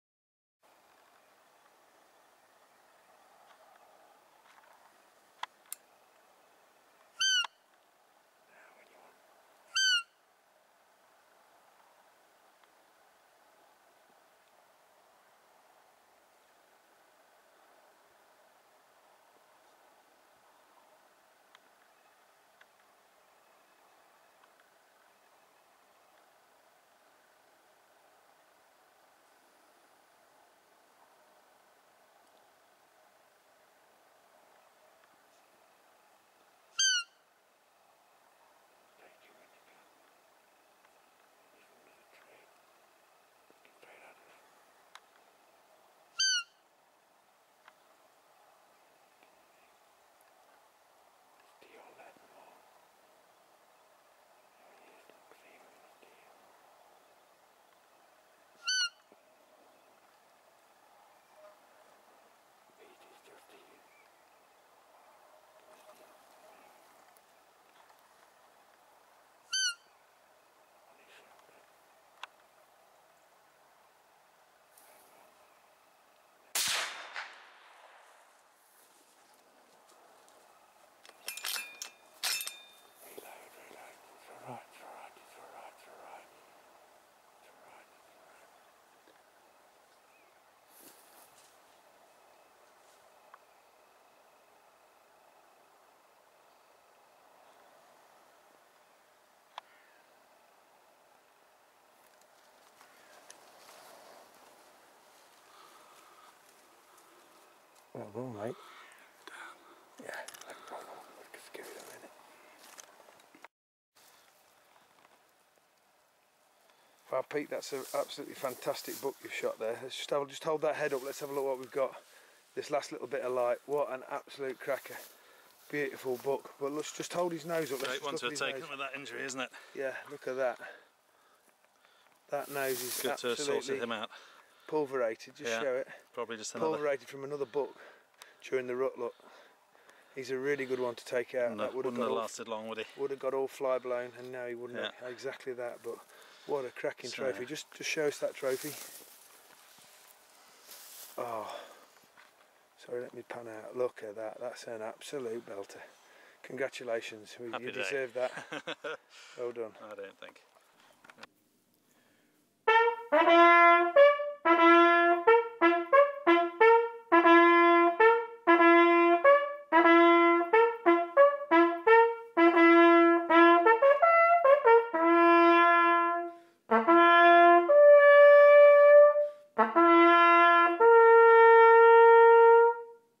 Peter's Cotswold Roebuck experience
Just when we think it is too late in the evening to get a buck, we find a real special one out in the wheat. A few calls and he comes to investigate. Its a quality buck but with a difference... he has a terrible injury that we do not realise, so in the end a mercy killing that has saved him from a very slow death...